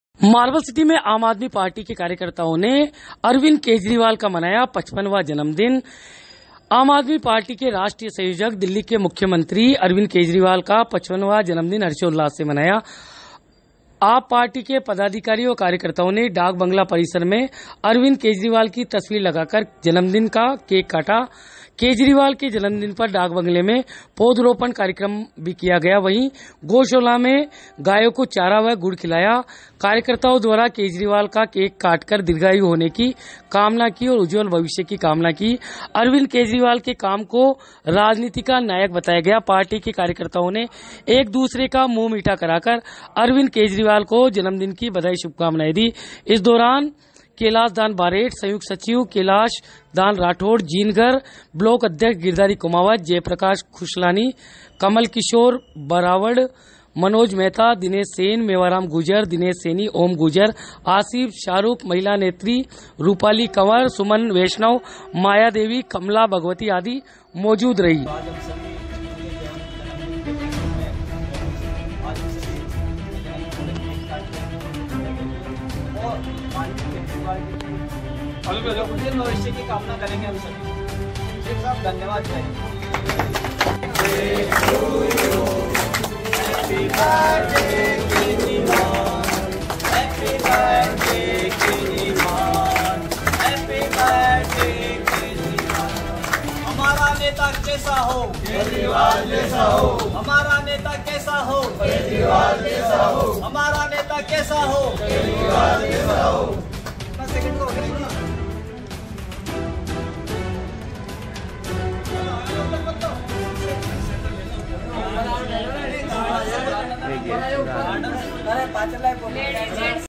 दिल्ली के मुख्यमंत्री आम आदमी पार्टी के सुप्रीमो अरविंद केजरीवाल का हर्षोल्लास से मनाया जन्मदिन
विमल गौड़
आम आदमी पार्टी ने मनाया अरविंद केजरीवाल का 55 वा जन्मदिन
आज आम आदमी पार्टी किशनगढ़ ने पार्टी के राष्ट्रीय संयोजक व दिल्ली के मुख्य मंत्री श्री अरविंद केजरीवाल जी 55 वा जन्मदिन  मनाया।
आज पार्टी के पदाधिकारी एवं कार्यकर्ता ने डाक बंगला परिसर मे श्री अरविंद केजरीवाल की तस्वीर लगाकर  उनके जन्मदिन का कैक काटा गया।
केजरीवाल के जन्मदिन पर डाक बंगला मे अशोका का पौधा भी लगाया गया। गायो को गुड़ व चारा खिलाया गया ।
 कार्यकर्ता द्वारा केजरीवाल का कैक  काटते हुए उनकी दीर्घायु की कामना की व उनके उज्जवल भविष्य की कामना  की गई। अरविंद केजरीवाल को काम की राजनीति का नायक बताया गया। पार्टी कार्यकर्ता ने एक दूसरे को मीठा मुँह कराया व अरविंद केजरीवाल को बधाई व शुभकामनाएं दी गई ll कार्यक्रम के दोरान मोजूद कार्यकर्ता मे इवेंट प्रभारी   कैलाश दान बारेठ,,  संयुक्त सचिव कैलाश चन्द राठौड़ जीनगर,ब्लॉक अध्यक्ष गिरधारी कुमावत,जय प्रकाश खुशलानी,कमल किशोर बरवाड,मनोज मेहता,दिनेश सेन,मेवाराम गुर्जर,,दिनेस सैनी,ओम गुर्जर, आसिफ,शाहरुख,महिला नेत्री मे रुपाली कंवर,सुमन वैष्णव,माया देवी,कमला,भगवती आदि ll